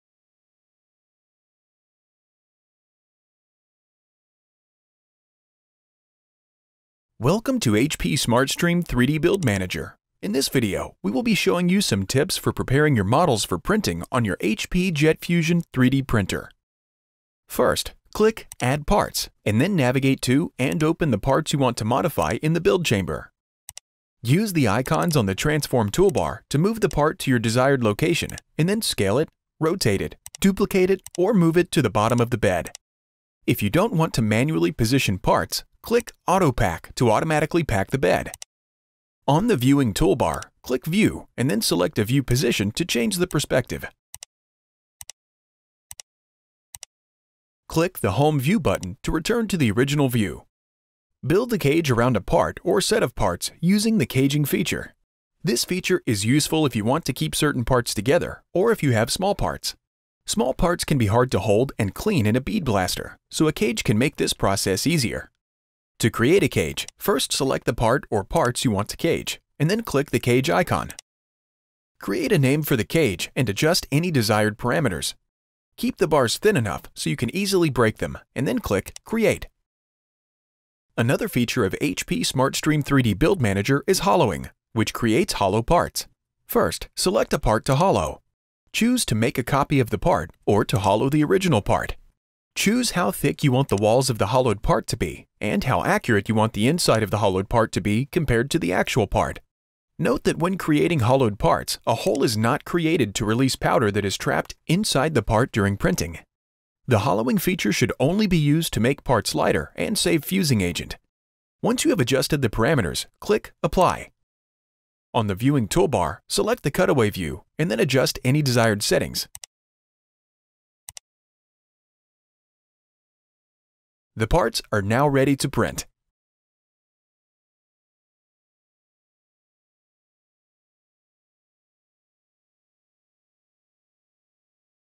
Prepare Models for 3D Printing - HP SmartStream 3D Build Manager | HP Jet Fusion
Learn how to prepare models for 3D printing using HP SmartStream 3D Build Manager for HP Jet Fusion 3D printers.

Chapters:
00:00 Introduction
00:17 Open the parts you want to modify
00:50 Build a cage
01:20 Use the hollowing feature
01:55 Adjust any desired settings

Follow these steps to prepare models for 3D printing using HP SmartStream 3D Build Manager for HP Jet Fusion 3D printers:
-First, click Add parts, and then navigate to and open the parts you want to modify in the build chamber.
-Use the icons on the Transform toolbar to move the part to your desired location, and then scale it, rotate it, duplicate it, or move it to the bottom of the bed.
-If you don’t want to manually position parts, click Auto-pack to automatically pack the bed.
-On the Viewing toolbar, click View and then select a view position to change the perspective.
-Click the Home view button to return to the original view.
-Build a cage around a part or set of parts using the caging feature.
-This feature is useful if you want to keep certain parts together or if you have small parts.
-Small parts can be hard to hold and clean in a bead blaster, so a cage can make this process easier.
-To create a cage, first select the part or parts you want to cage, and then click the Cage icon.
-Create a name for the cage and adjust any desired parameters.
-Keep the bars thin enough so you can easily break them, and then click Create.
-Another feature of HP SmartStream 3D Build Manager is Hollowing, which creates hollow parts.
-First, select a part to hollow.
-Choose to make a copy of the part or to hollow the original part.
-Choose how thick you want the walls of the hollowed part to be, and how accurate you want the inside of the hollowed part to be compared to the actual part.
-Note that when creating hollowed parts, a hole is not created to release powder that is trapped inside the part during printing.
-The hollowing feature should only be used to make parts lighter and save fusing agent.
-Once you have adjusted the parameters, click Apply.
-On the Viewing toolbar, select the Cutaway view, and then adjust any desired settings.
-The parts are now ready to print.

How to Prepare Models for 3D Printing - HP SmartStream 3D Build Manager | HP Jet Fusion | HP